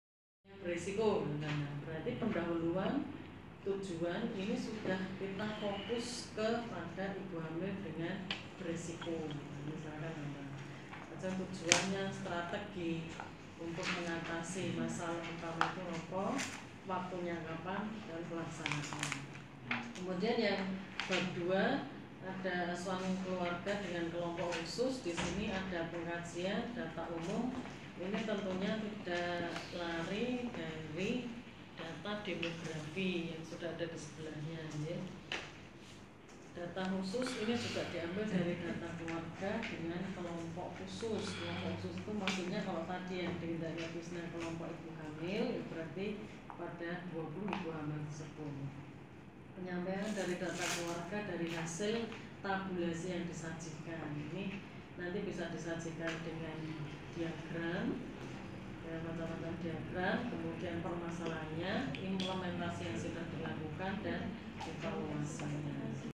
Ketua PKMD Stikesmus ketika memimpin Rapat
Ketua PKMD Stikesmus "Tri Yuniarti" ketika memimpin Rapat koordinasi PKMD yang akan dilaksanakan diwilayah cempogo boyolali...